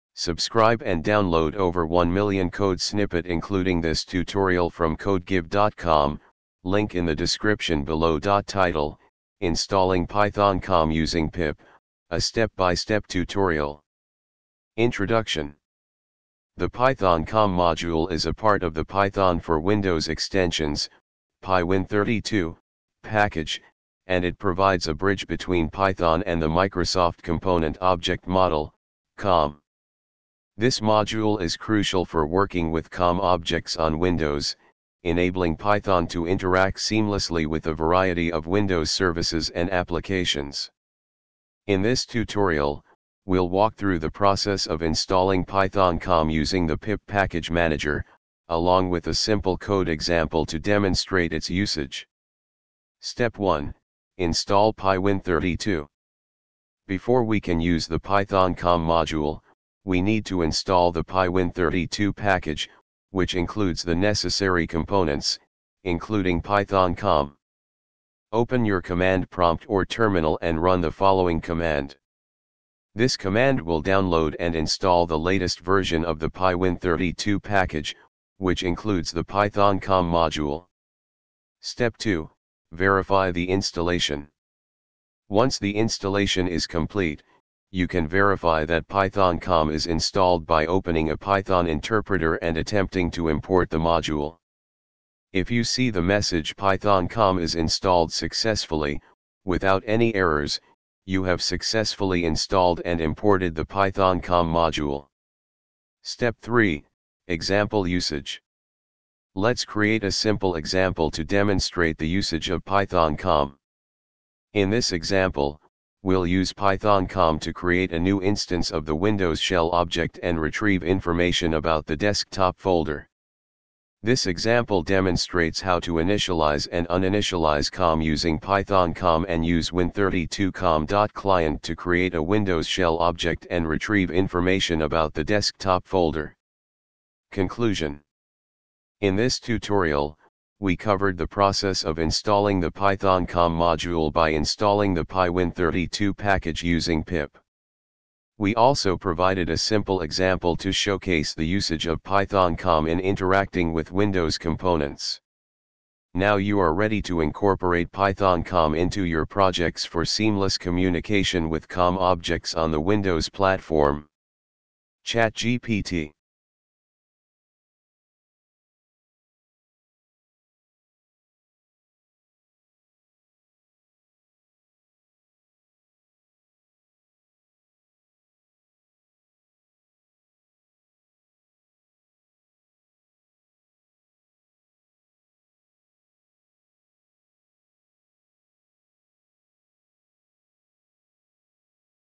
pip install pythoncom
Title: Installing pythoncom using pip - A Step-by-Step Tutorial
Introduction:
The pythoncom module is a part of the Python for Windows extensions (pywin32) package, and it provides a bridge between Python and the Microsoft Component Object Model (COM). This module is crucial for working with COM objects on Windows, enabling Python to interact seamlessly with a variety of Windows services and applications. In this tutorial, we'll walk through the process of installing pythoncom using the pip package manager, along with a simple code example to demonstrate its usage.
Step 1: Install pywin32
Before we can use the pythoncom module, we need to install the pywin32 package, which includes the necessary components, including pythoncom. Open your command prompt or terminal and run the following command:
This command will download and install the latest version of the pywin32 package, which includes the pythoncom module.
Step 2: Verify the installation
Once the installation is complete, you can verify that pythoncom is installed by opening a Python interpreter and attempting to import the module:
If you see the message "pythoncom is installed successfully!" without any errors, you have successfully installed and imported the pythoncom module.
Step 3: Example Usage
Let's create a simple example to demonstrate the usage of pythoncom. In this example, we'll use pythoncom to create a new instance of the Windows Shell object and retrieve information about the desktop folder.
This example demonstrates how to initialize and uninitialize COM using pythoncom and use win32com.client to create a Windows Shell object and retrieve information about the desktop folder.
Conclusion:
In this tutorial, we covered the process of installing the pythoncom module by installing the pywin32 package using pip. We also provided a simple example to showcase the usage of pythoncom in interacting with Windows components. Now you are ready to incorporate pythoncom into your projects for seamless communication with COM objects on the Windows platform.
ChatGPT